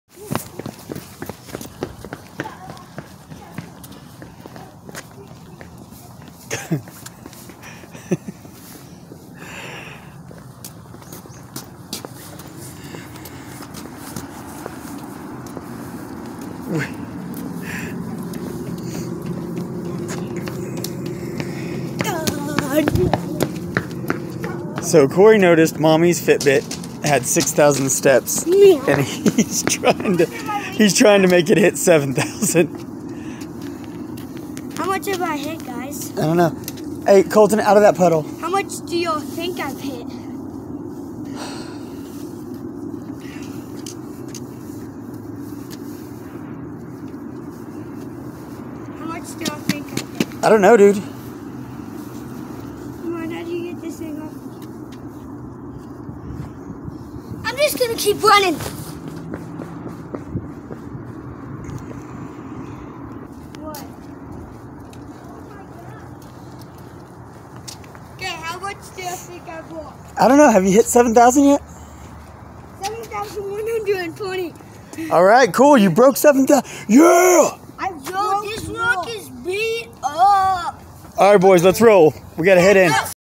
Corey II Noticed Mommy's FitBit was Over 6k Steps ... ~ Determined to Break 7k for Her 💪💯👊
My beast mode boy... I'm telling you ...Being Bayless, we know no other way
and ding that Bell 🔔 to get your notifications.
You don't want to miss any of this...